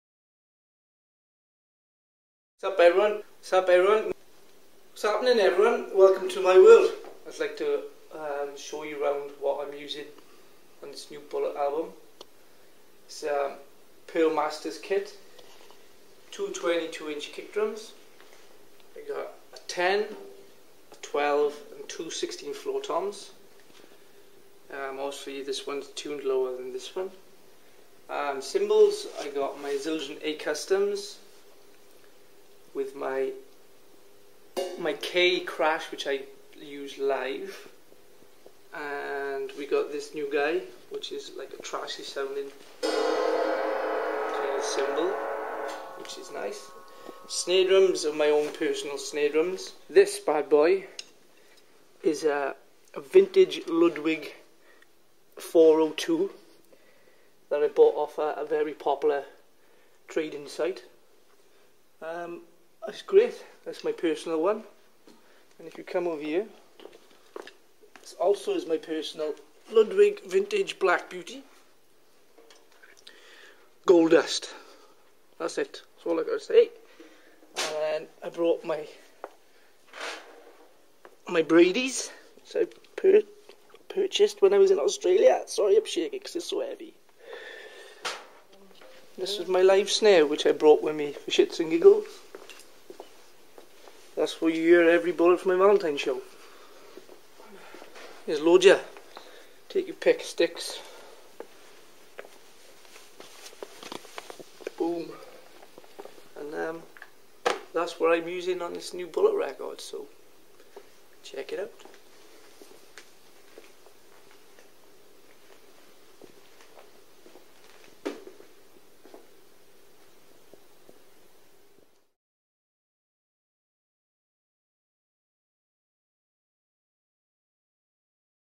In the Studio: Moose's Drum Set-up
More footage from the studio where Bullet For My Valentine just finished up recording their upcoming 4th album.